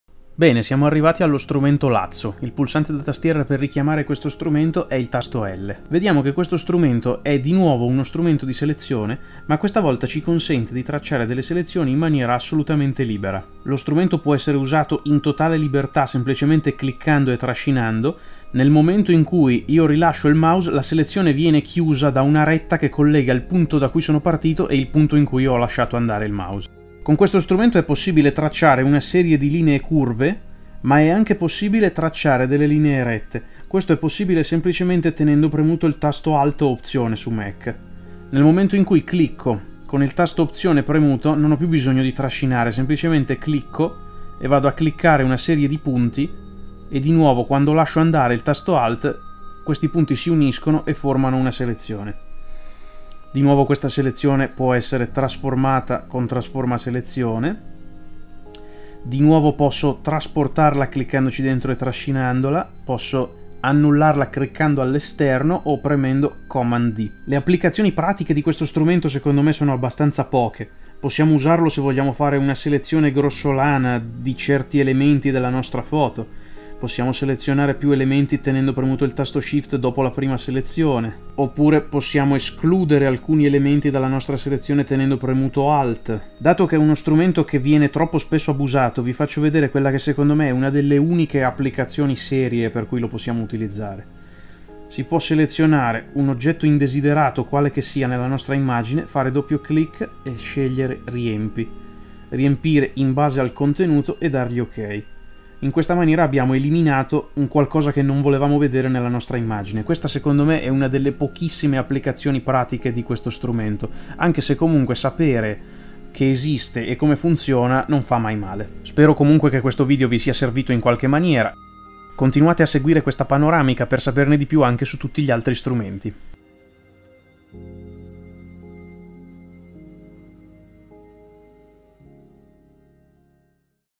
PHOTOSHOP - Strumento Lazo
Selezioni personalizzate con lo Strumento Lazzo!

Ciao a tutti, appassionati di fotoritocco e creatività con Photoshop! Nel mio ultimo video, ci immergeremo nel potente mondo del "Strumento Lazo" di Photoshop. Questo strumento è uno dei preferiti tra gli artisti digitali e i professionisti del fotoritocco ed è ampiamente utilizzato per creare selezioni personalizzate nelle tue immagini.

Durante questo tutorial completo, ti guiderò attraverso tutte le funzionalità e le tecniche chiave per utilizzare il "Strumento Lazo" con maestria. Imparerai a creare selezioni libere, poligonali o magnetiche, a seconda delle tue esigenze specifiche. Scoprirai come applicare selezioni precise, ritagliare parti delle tue immagini e persino combinare il "Lazo" con altri strumenti di selezione per risultati sorprendenti.

Non importa se sei un principiante o un esperto di Photoshop, questo video ti fornirà preziose informazioni su come sfruttare al meglio il "Strumento Lazo" per migliorare il tuo fotoritocco. Preparati a diventare un vero mago della selezione con il "Strumento Lazo" di Photoshop. Buon fotoritocco a tutti!